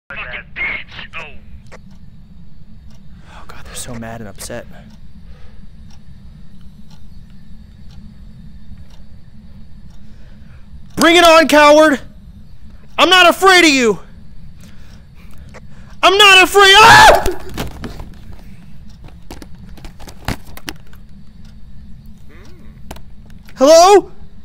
Sodapoppin says he is not afraid, but it seems like he is
Sodapoppin says he is not afraid, but it seems like he is after all